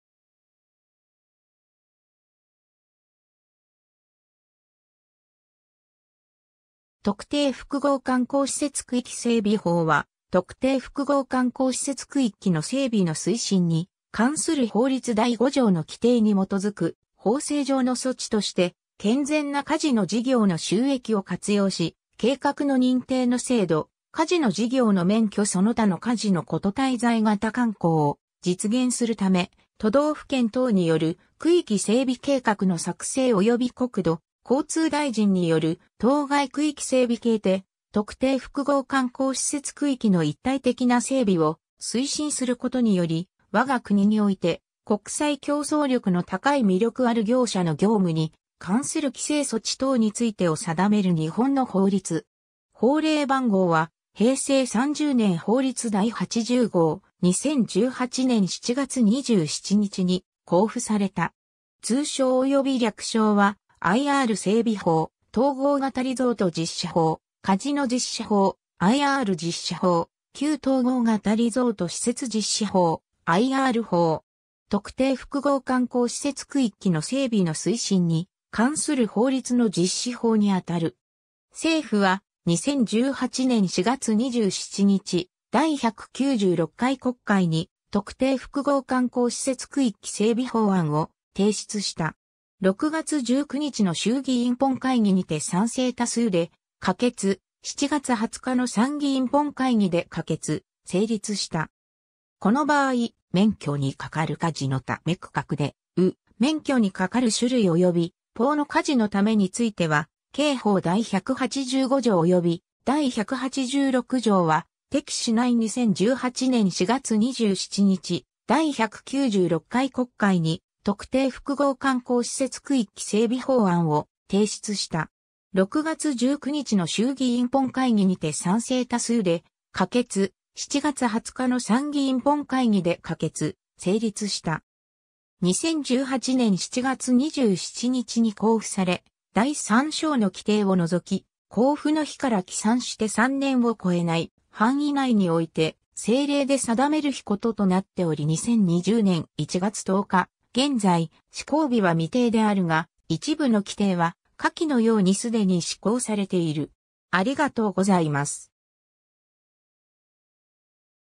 特定複合観光施設区域整備法
特定複合観光施設区域整備法（とくていふくごうかんこうしせつくいきせいびほう）は、特定複合観光施設区域の整備の推進に関する法律第5条の規定に基づく法制上の措置として健全なカジノ事業の収益を活用し計画の認定の制度、カジノ事業の免許その他のカジノ事滞在型観光を実現するため、都道府県等による区域整備計画の作成及び国土交通大臣による当該区域整備計て特定複合観光施設区域の一体的な整備を推進することにより、我が国において国際競争力の高い魅力ある業者の業務に関する規制措置等についてを定める日本の法律。
法令番号は平成30年法律第80号、2018年（平成30年）7月27日に交付された。
通称および略称は、IR整備法、統合型リゾート実施法、カジノ実施法、IR実施法、9統合型リゾート施設実施法、IR法。
 特定複合観光施設区域の整備の推進に関する法律の実施法にあたる。
 政府は、2018年（平成30年）4月27日、第196回国会に特定複合観光施設区域整備法案を提出した。
6月19日の衆議院本会議にて賛成多数で可決、7月20日の参議院本会議で可決、成立した。
 この場合、免許に係るカジノ⾏為区画で⾏う、免許に係る種類及び⽅法のカジノ⾏為については、刑法第185条（賭博）及び第186条（常習賭博及び賭博場開張等図利）は適⽤しない 2018年（平成30年）4月27日、第196回国会に特定複合観光施設区域整備法案を提出した。
6月19日の衆議院本会議にて賛成多数で可決、7月20日の参議院本会議で可決、成立した。
 2018年（平成30年）7月27日に公布され、第3章の規定 を除き、「公布の日から起算して3年を超えない範囲内において政令で定める日」（附則1条本文）こととなっており2020年1月10日現在、施行日は未定であるが、一部の規定は、下記のようにすでに施行されている。